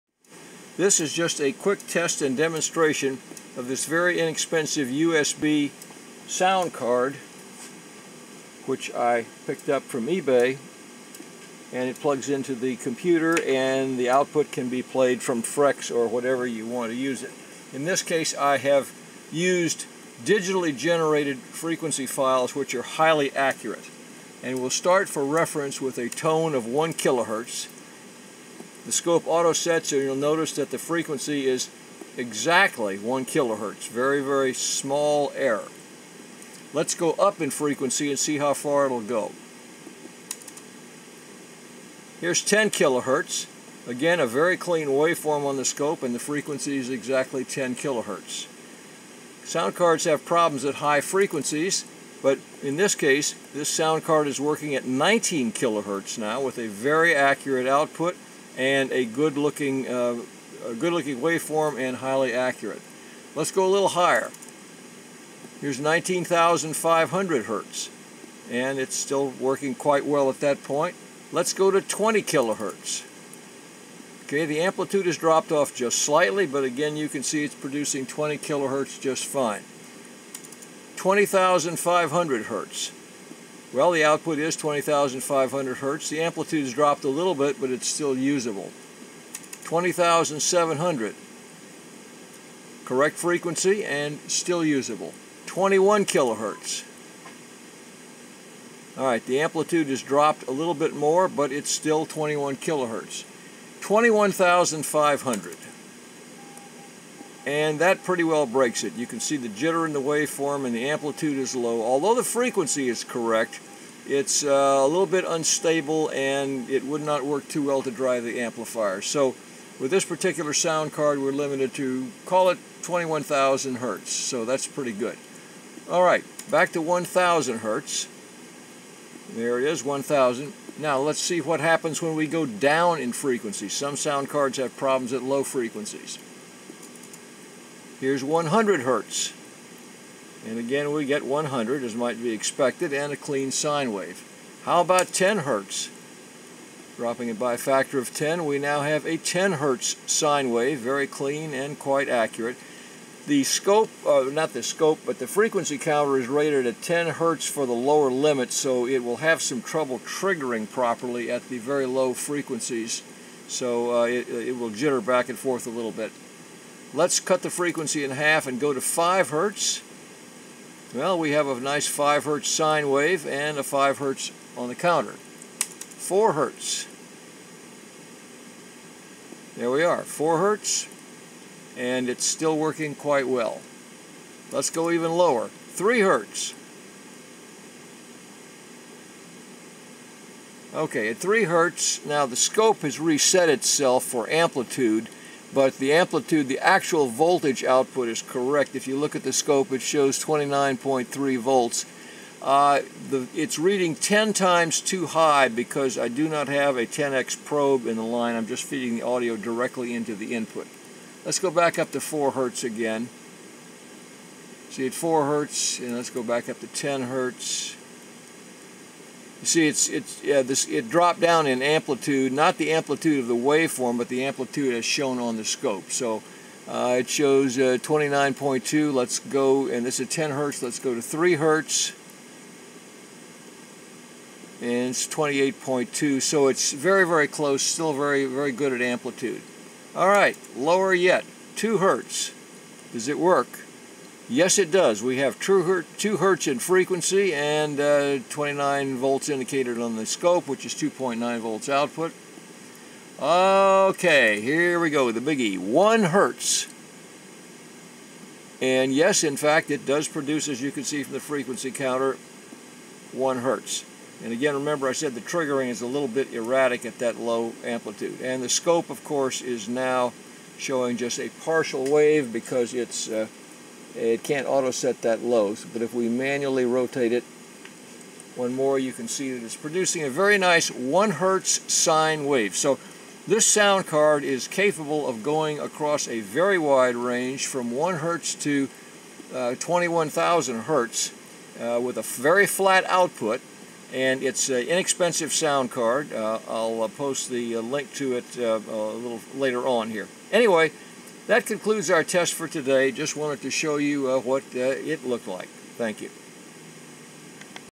Inexpensive USB Sound Card Test for Frequency Response and Accuracy
Here I am testing a $15.00 USB sound card adapter of the type readily available from eBay and other sources.

I wanted to see how accurate these devices are and whether or not they would be usable with FreX16 to drive the SPA5 amplifier.  As it turns out, the answer is, yes, they work quite well.

I have tested several of these USB sound card adapters from different manufacturers and they all seem to work about the same, which is to say, they work very well especially considering the low price.

Near the end of the video, I mention that I will post a link to this adapter, but because the listings change so often, I decided not to add a link.  Just do an internet search for "USB SOUND CARD ADAPTER" if you want to find them.